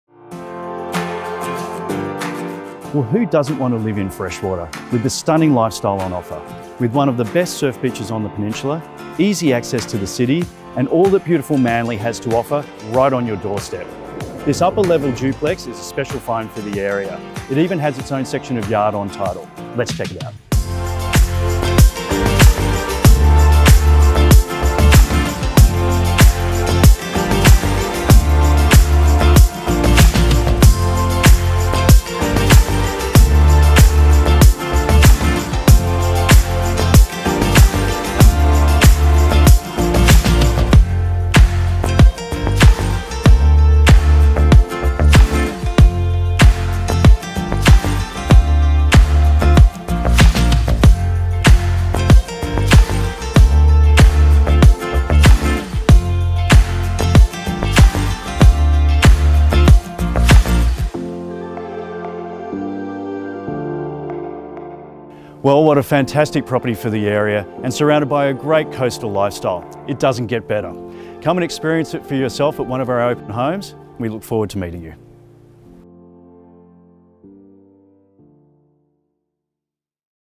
SOLD | 2/16 Wyuna Avenue, Freshwater | Steve Lambley
Sunlit upper duplex by the beach

Sunlit and charming, this north-facing upper duplex will have your heart the minute you walk in the door.